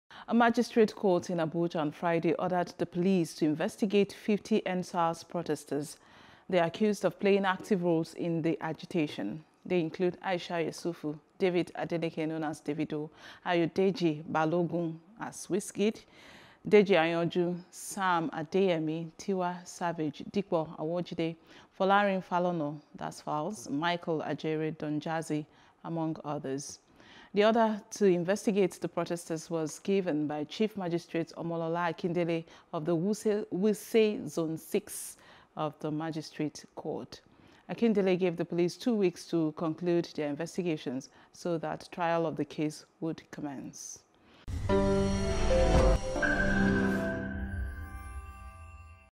Endsars: Court Orders Police to Probe Davido, Wizkid, Falz, And Others (News | Nigeria)
A Magistrate Court in Abuja on Friday ordered the Police to investigate 50 End SARS protesters.